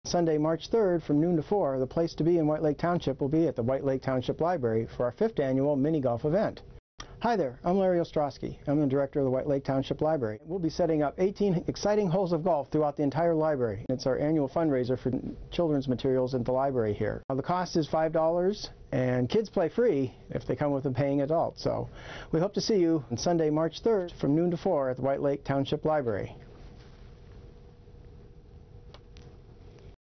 White Lake Township Library Mini-Golf Returns!
Golf with the family! March 3 12 - 4 pm at the White Lake Townhsip Library
Greens fees: $5 for adults, kids free with paying adult!
All proceeds go to purchase new materials for the Children's department!